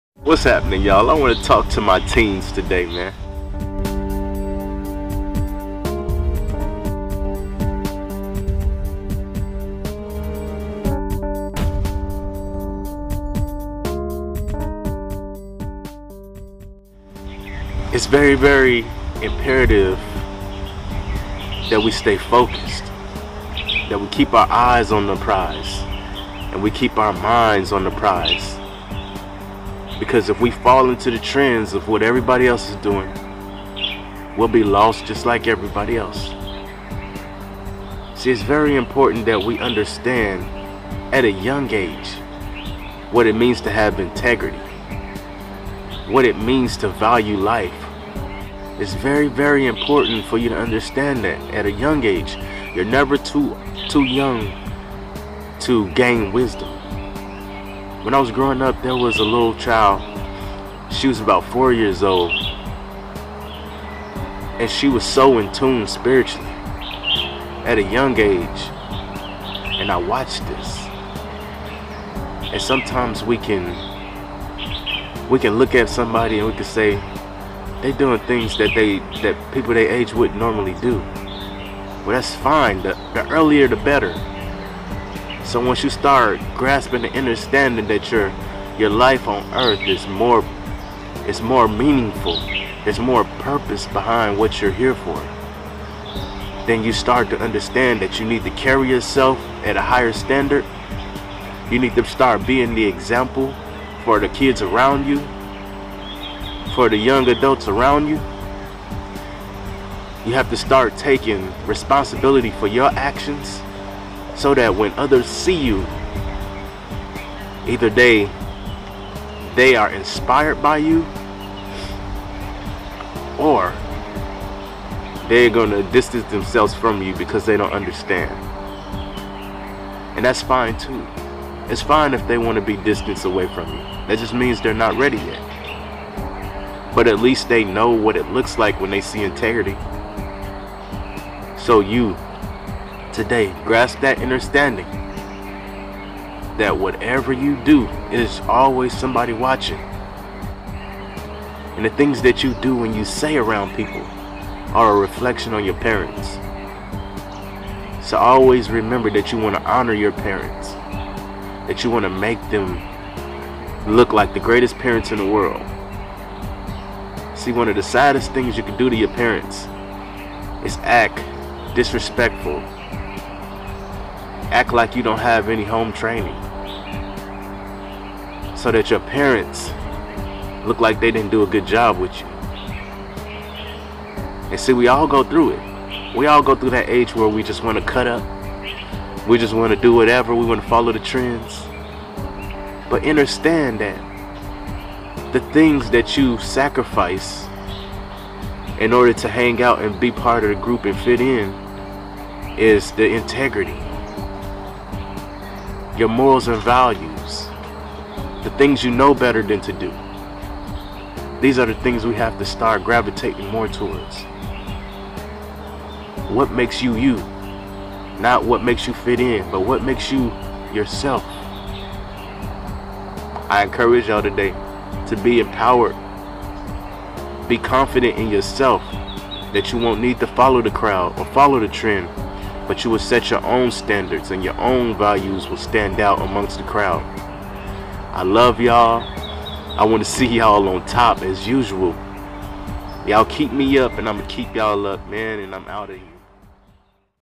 Message to the Youth - The Sequel
Let's show the world that we are not to young to have integrity. We have good morals and values as young men and women.